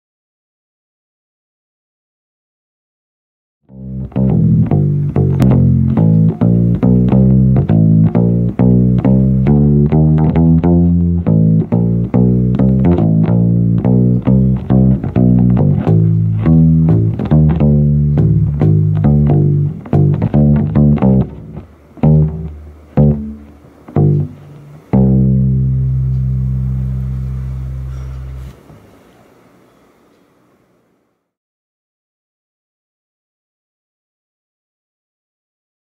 Colin D'Cruz - Kala U-Bass
My first take on this pocket sized bass, more bass less space!